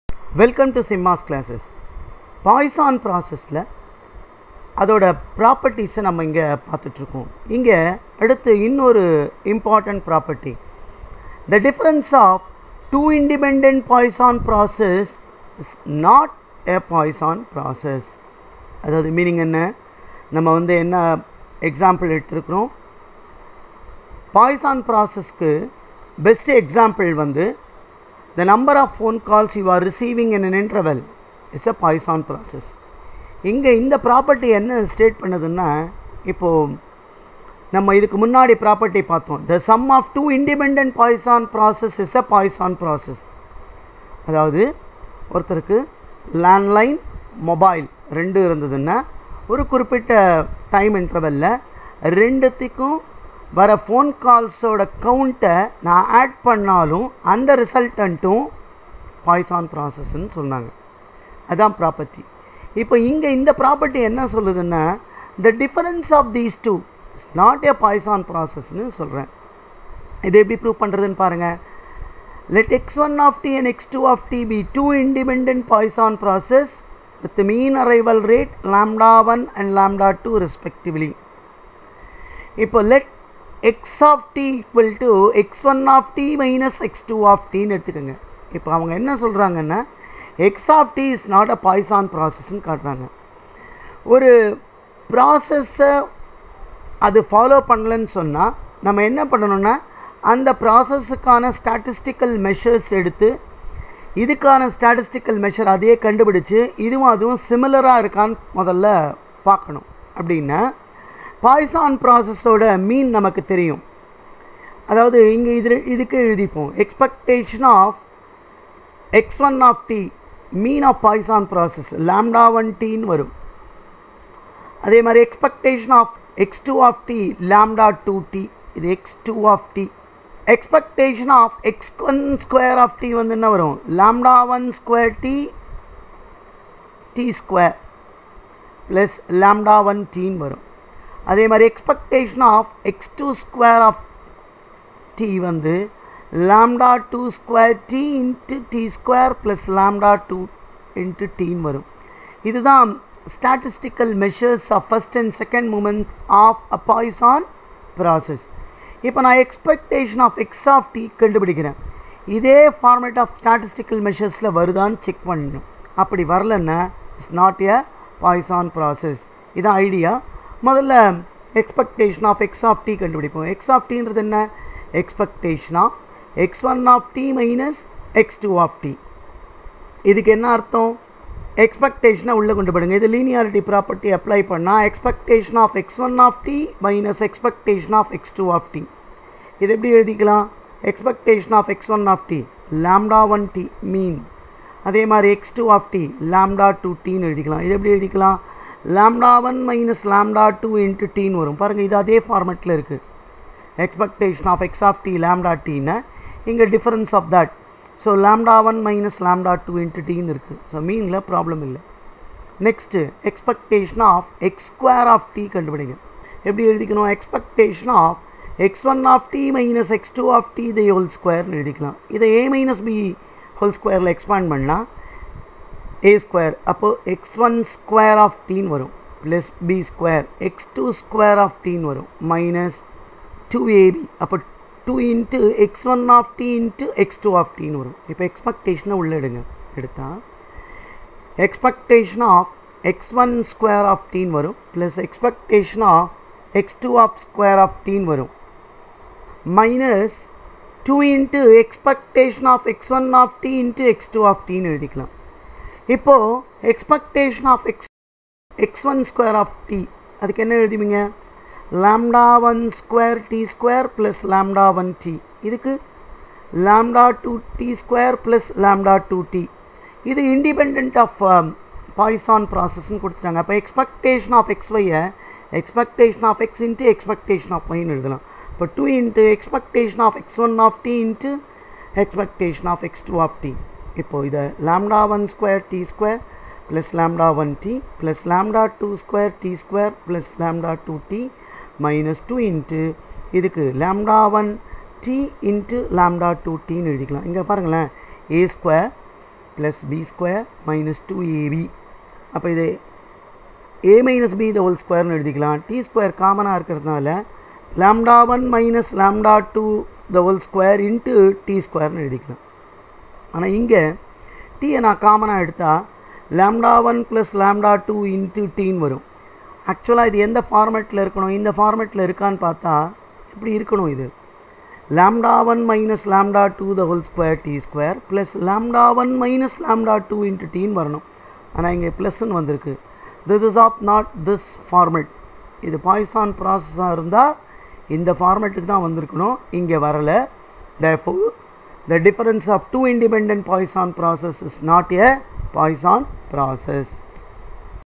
Poisson_process || The difference of two independent Poisson processes is not a Poisson process.
If f(x,y) = x+y,   0 less than x, y less than 1. find the pdf of the random variable U=XY
Continuous _rv X  has the pdf  f(x)=kx^4,  Find prob.of x greater than -1by2 given x less than -1by4